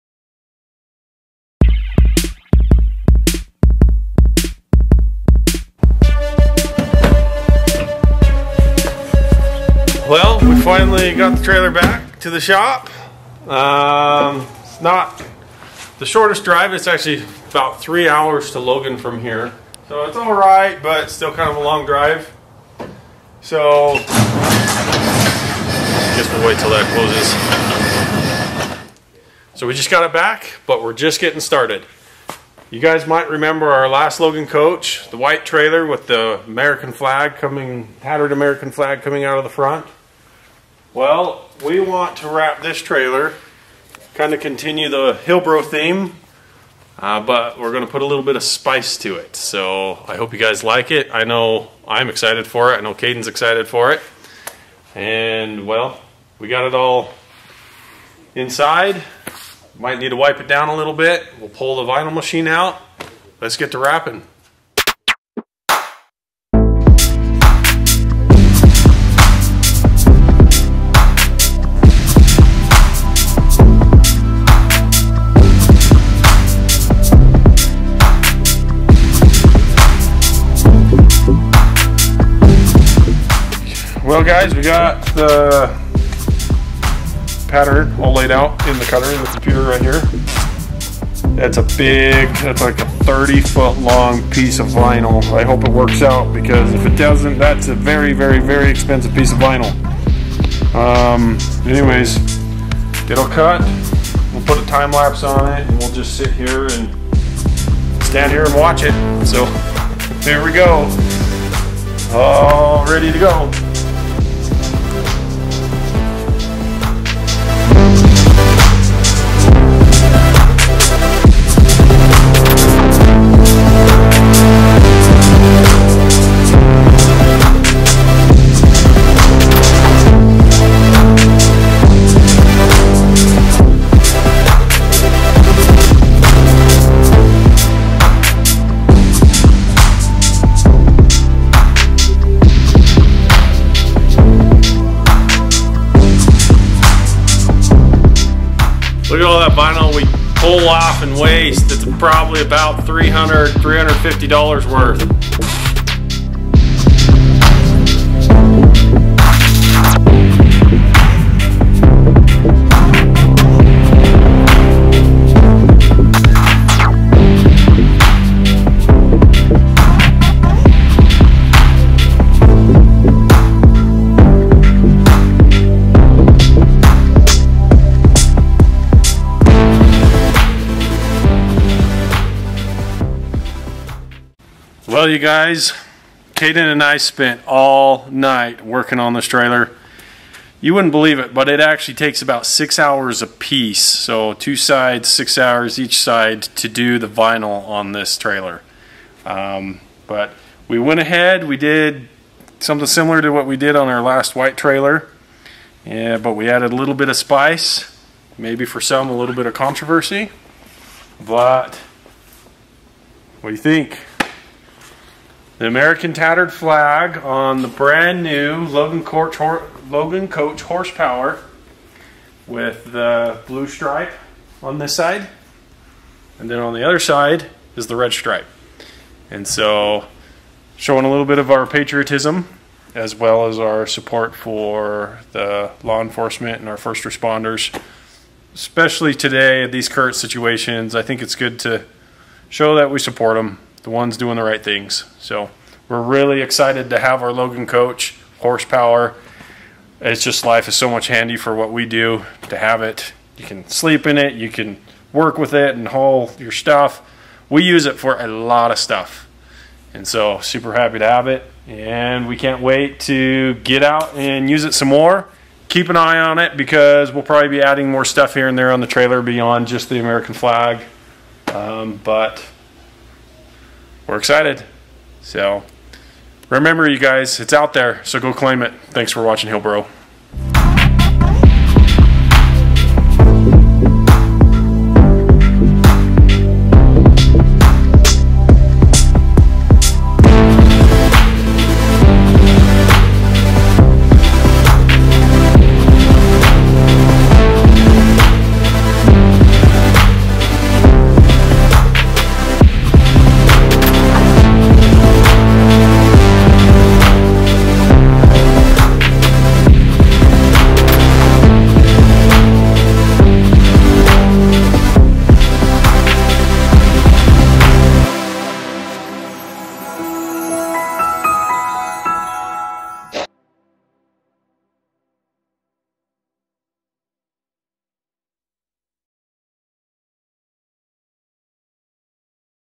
NICE TRAILER WRAP Logan Coach HorsePower
In this video we put the tattered American flag wrap on the new Logan Coach Horse Power trailer.